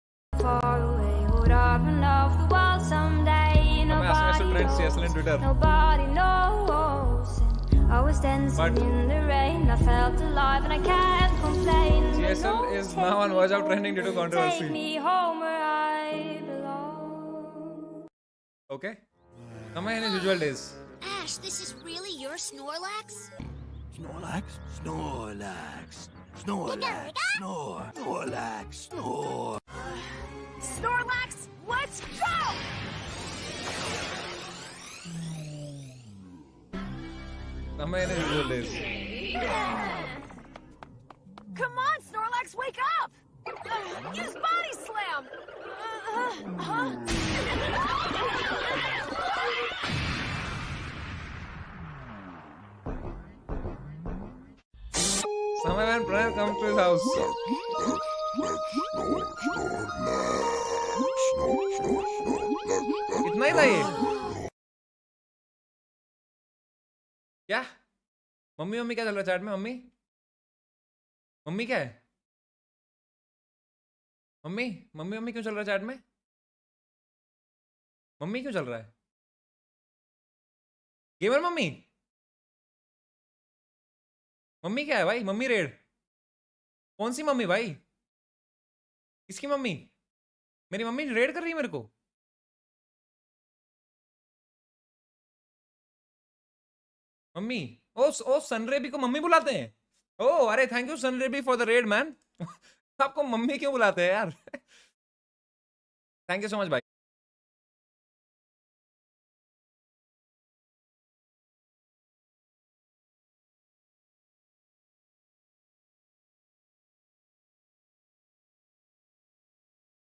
Samay Raina Reacts to Sunraybee Meme (Raid)
Samay Audience started spamming mummy and Samay thought Sunraybee raid